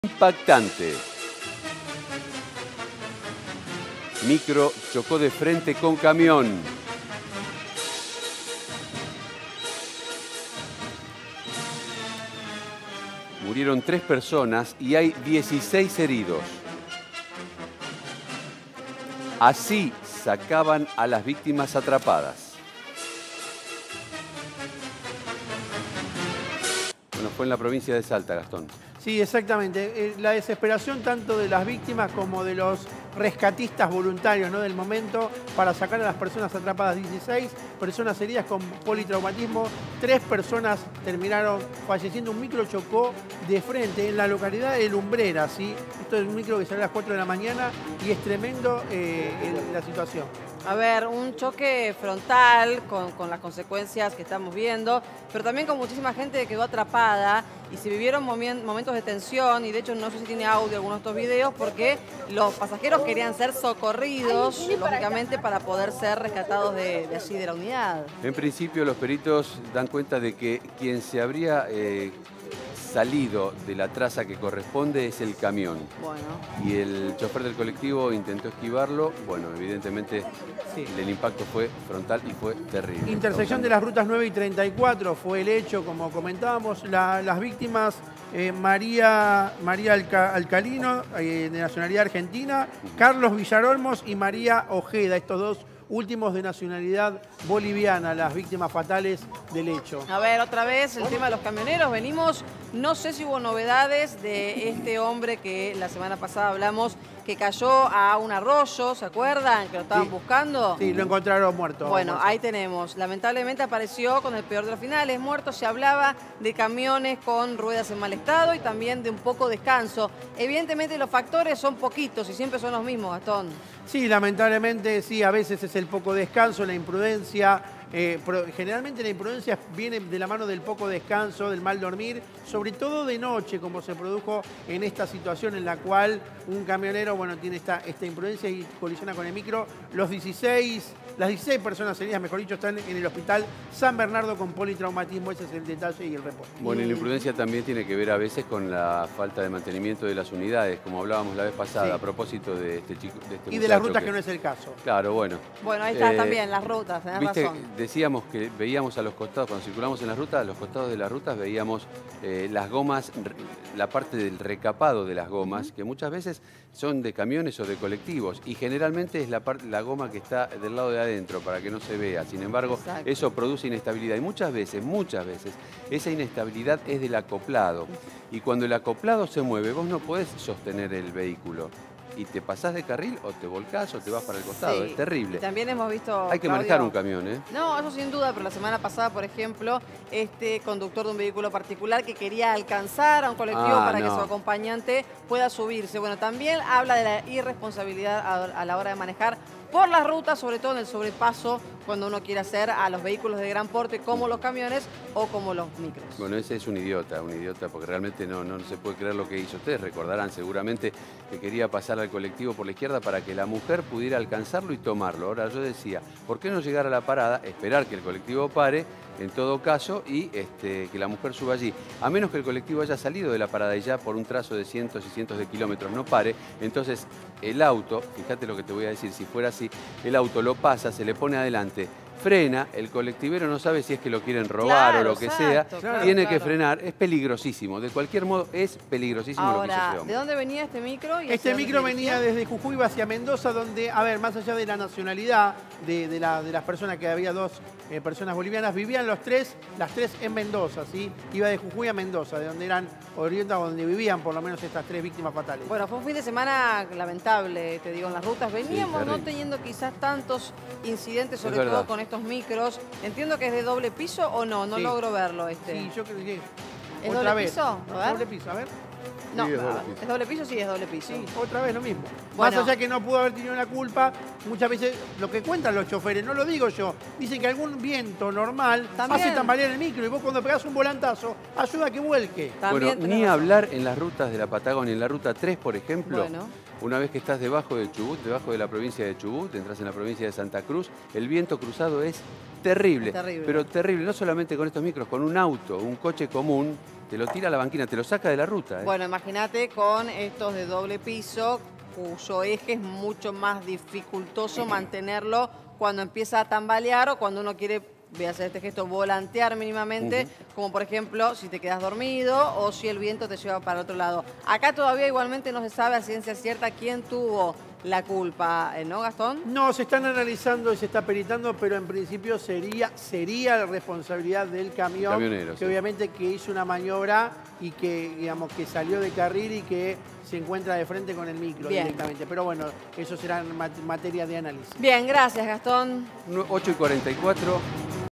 Accidente fatal en Salta: así sacaban a las víctimas atrapadas
Un micro de pasajeros chocó de frente contra un camión a la altura de la localidad de Lumbreras dejando como saldo tres personas muertas y dieciséis heridos.

Las imágenes, que fueron registradas con un teléfono celular, muestran el momento en el que intentan rescatar a las personas de entre los hierros retorcidos.

Dejanos tu mensaje en la caja de comentarios y si querés podés dejar también un saludo, te leemos y te respondemos.